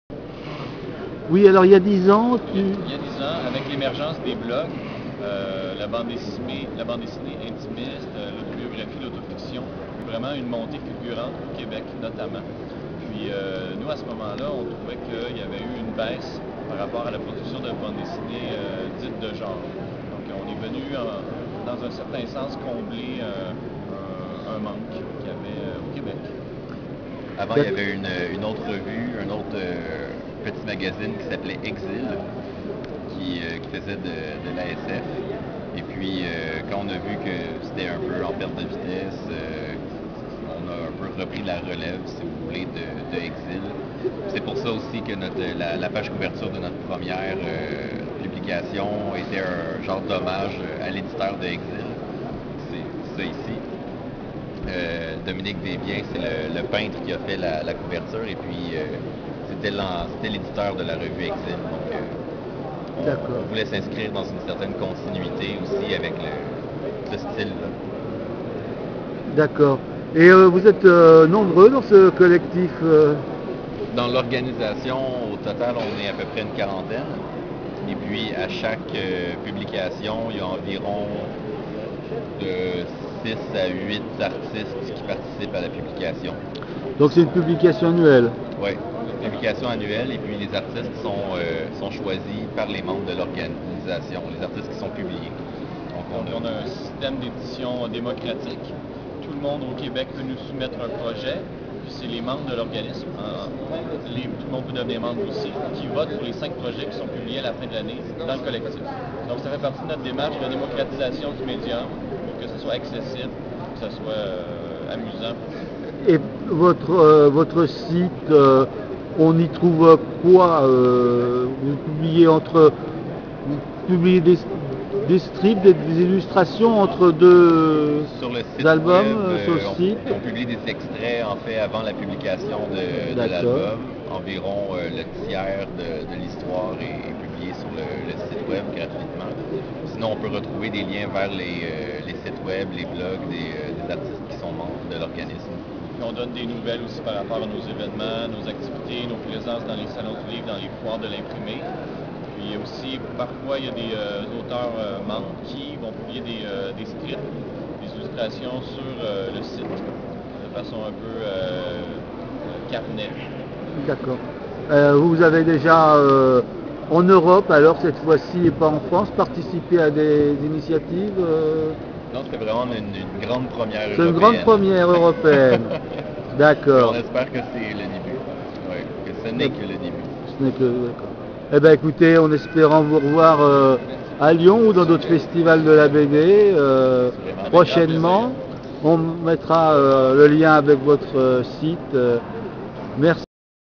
Front Froid Quebec1
Rencontre avec les animateurs du collectif quebecquois Front Froid